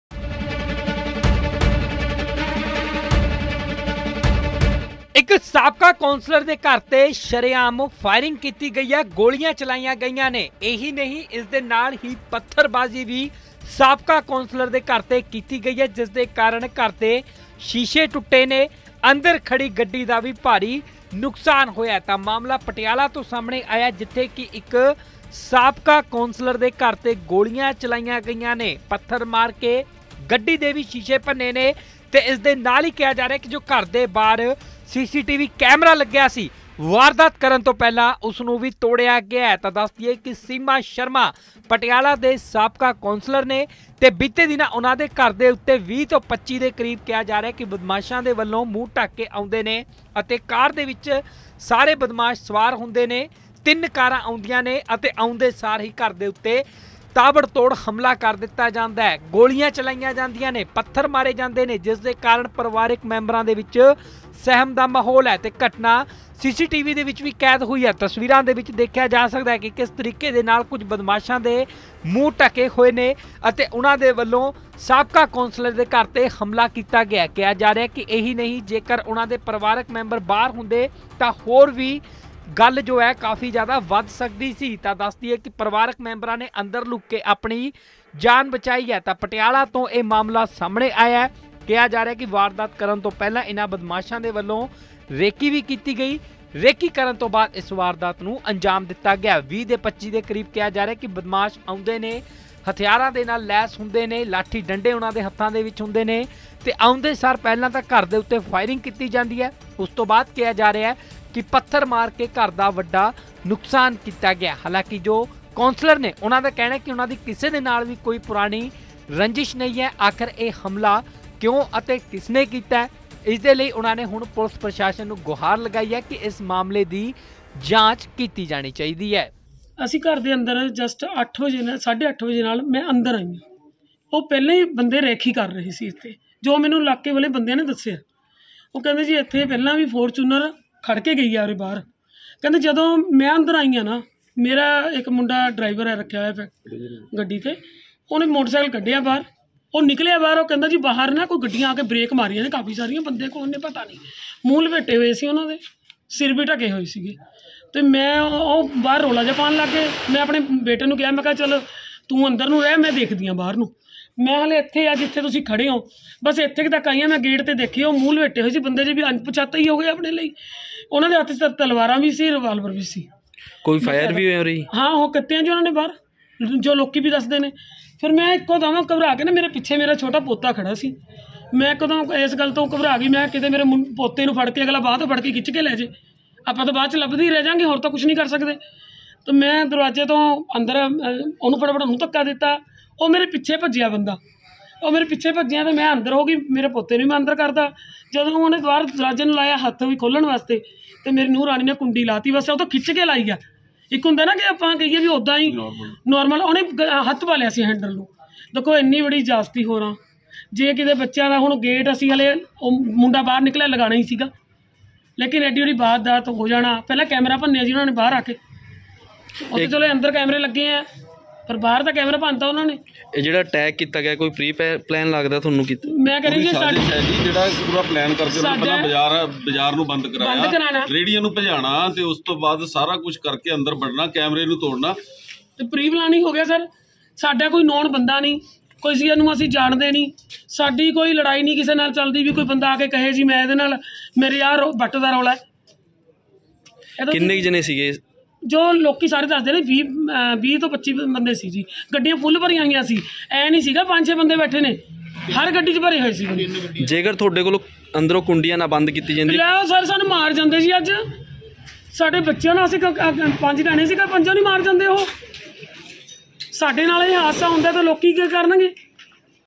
Firing In Patiala | ਸਾਬਕਾ ਕੌਂਸਲਰ ਦੇ ਘਰ 'ਤੇ ਚਲਾਈਆਂ ਗੋਲੀਆਂ
ਪਟਿਆਲਾ ਦੀ ਸਾਬਕਾ ਕੌਂਸਲਰ ਸੀਮਾ ਸ਼ਰਮਾ ਦੇ ਘਰ 15 ਤੋਂ 20 ਅਣਪਛਾਤੇ ਲੋਕਾਂ ਵੱਲੋਂ ਹਮਲਾ ਕੀਤਾ ਗਿਆ। ਉਨ੍ਹਾਂ ਦੇ ਘਰ 'ਤੇ ਪੱਥਰਬਾਜ਼ੀ ਅਤੇ ਫਾਇਰਿੰਗ ਵੀ ਕੀਤੀ ਗਈ, ਜਿਸ ਕਾਰਨ ਘਰਾਂ ਦੇ ਸ਼ੀਸ਼ੇ ਅਤੇ ਵਾਹਨਾਂ ਦੇ ਸ਼ੀਸ਼ੇ ਟੁੱਟ ਗਏ। ਇਹ ਸਾਰੀ ਘਟਨਾ ਸੀਸੀਟੀਵੀ ਵਿਚ ਕੈਦ ਹੈ।